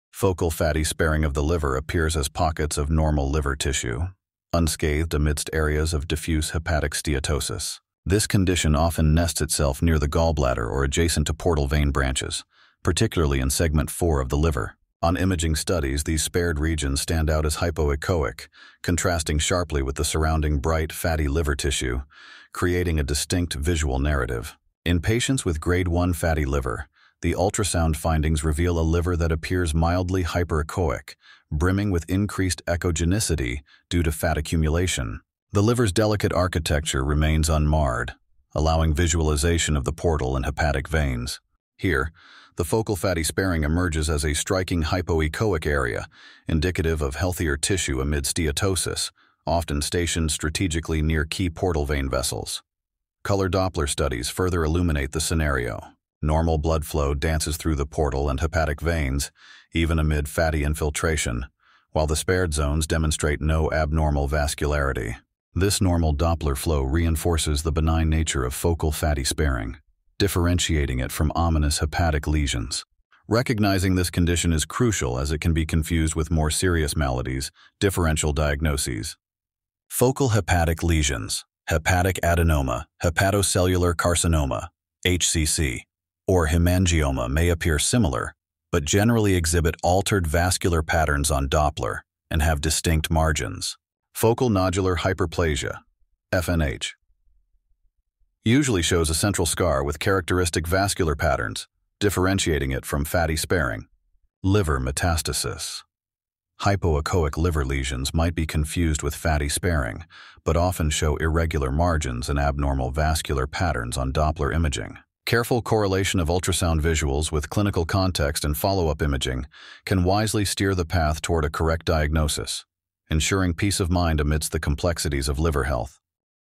Understanding Focal Fatty Sparing in Fatty Liver: Ultrasound & color Doppler Insights
Focal fatty sparing of the liver refers to regions of normal liver tissue that remain unaffected by diffuse fatty infiltration, commonly found near the gallbladder or portal vein branches. In cases of grade 1 fatty liver, the liver appears mildly hyperechoic due to fat deposition, but focal fatty sparing shows up as a hypoechoic area within this bright liver tissue. These spared regions have a normal echotexture compared to the surrounding fatty liver. On ultrasound, the liver’s architecture remains intact, and portal vein walls are still visible. Color Doppler typically shows normal blood flow in the portal and hepatic veins, with no abnormal vascularity in the focal fatty sparing areas.

Differential diagnoses include focal hepatic lesions like adenoma, hepatocellular carcinoma (HCC), and liver metastasis, which may appear similar but differ in vascular patterns. Importantly, focal fatty sparing is a benign finding and does not require treatment. Accurate diagnosis is crucial to avoid confusion with more serious liver conditions.